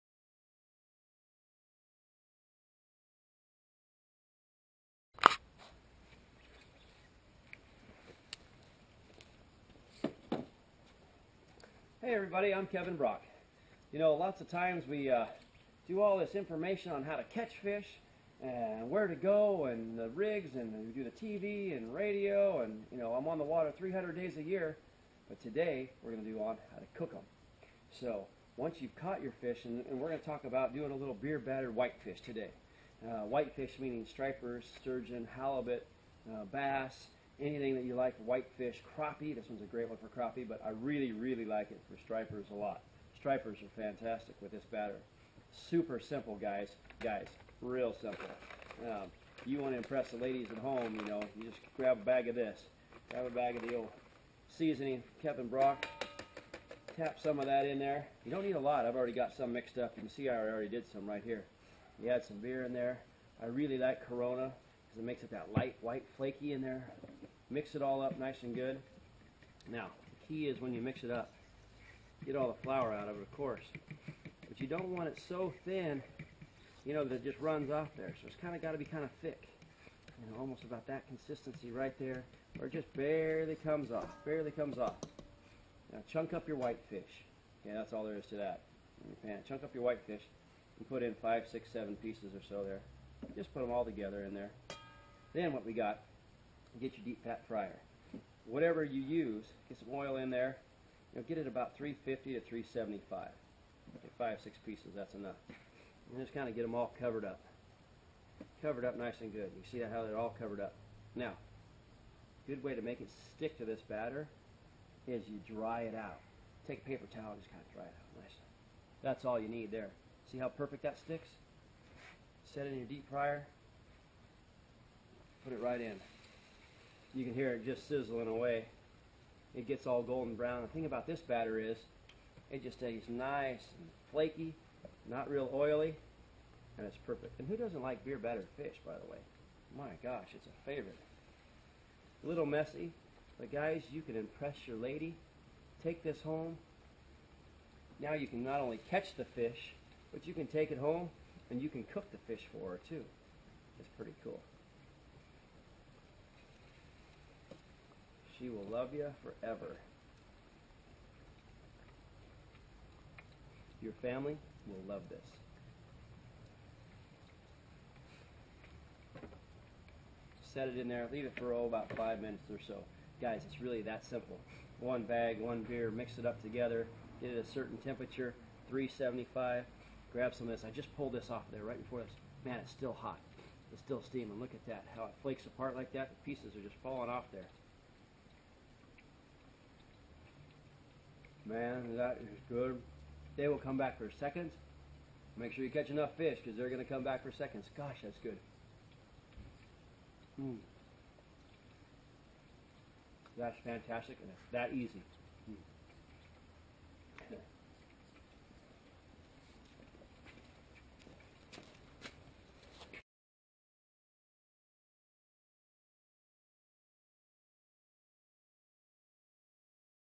Beer Batter Cooking with Pro Guide Kevin Brock
There's nothing like the delicious taste of fresh fish dipped into Kevin Brock's beer batter and fried to crispy golden perfection. This video shows you how.